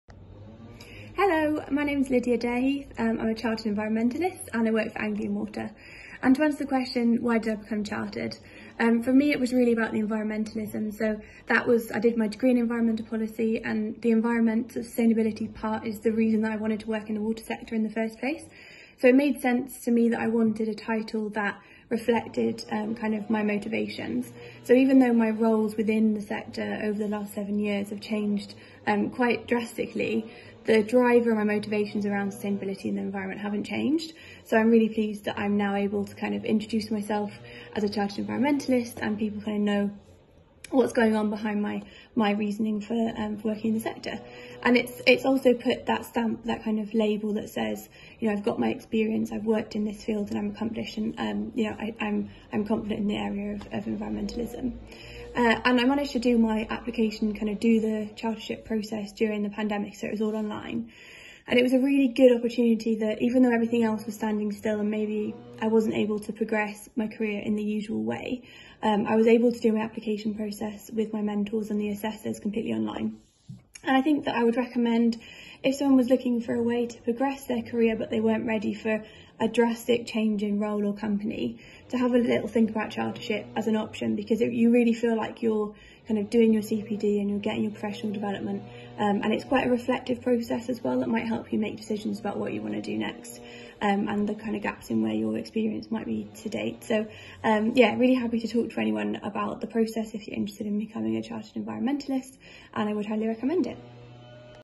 Become a Chartered Environmentalist (CEnv) with IWater
Lydia Dareheath CEnv, Public Policy Advisor, Anglian Water, explains why she chose to go through IWater for her Chartered Environment Registration.

IWater is licensed to award professional registrations on behalf of the Society for the Environment.The registration has three sections:

  Chartered Environmentalist (CEnv)
 Registered Environmental Practitioner (IREnvP)
  Registered Environmental Technician (REnvTech)

If you are interested in displaying your commitment to good environmental practice and want to further your career, why not consider becoming a Chartered Environmentalist through IWater?